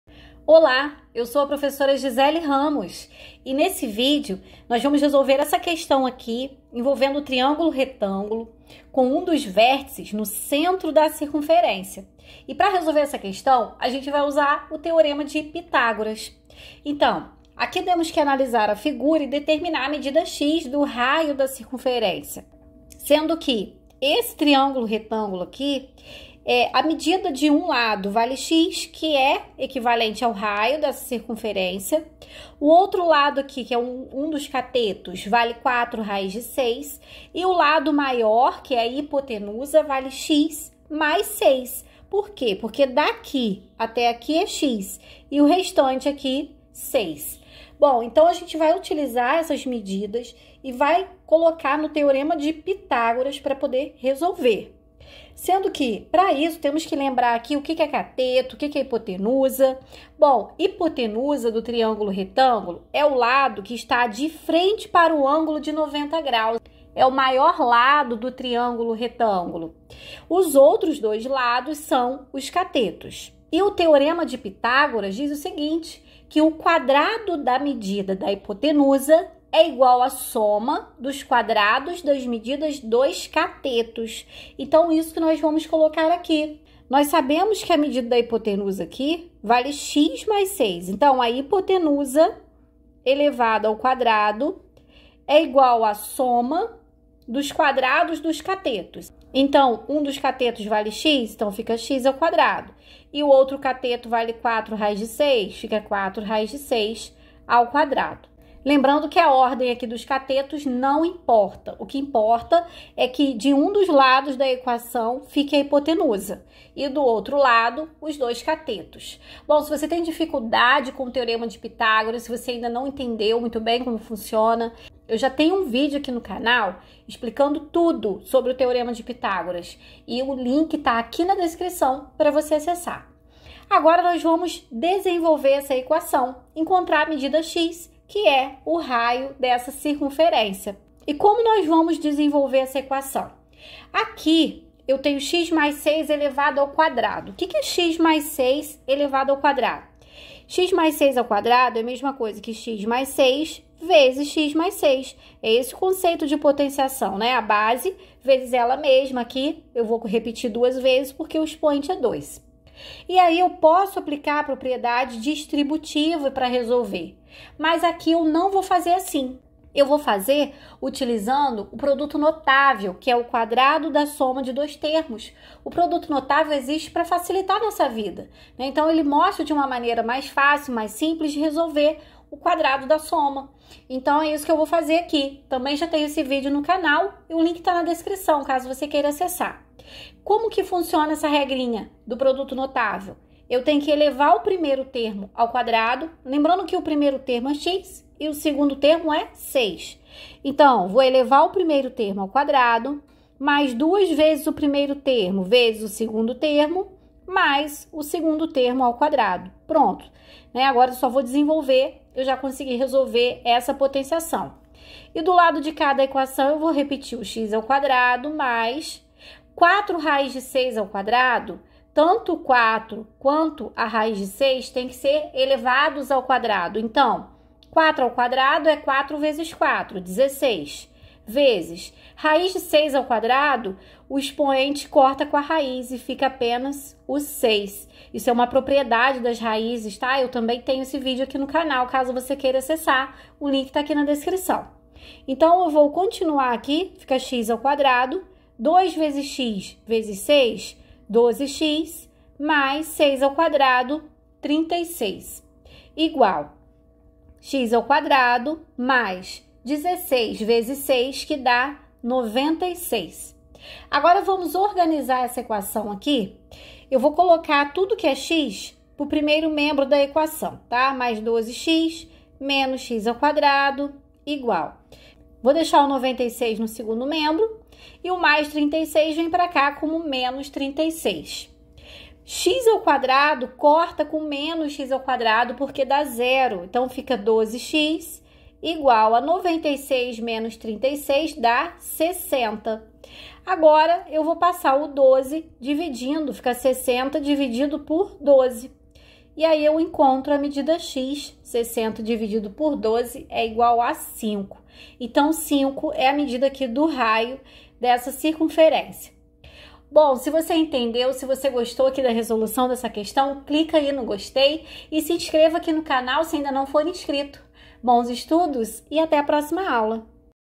QUAL É A MEDIDA DO RAIO DESSA CIRCUNFERÊNCIA? 👩‍🏫 GISELE RAMOS
Olá! Nesta aula vamos resolver uma questão onde precisamos determinar a medida do raio de uma circunferência. Para isso utilizaremos alguns conceitos matemáticos tais como teorema de Pitágoras,  o quadrado da soma de dois termos e uma propriedade dos radicais.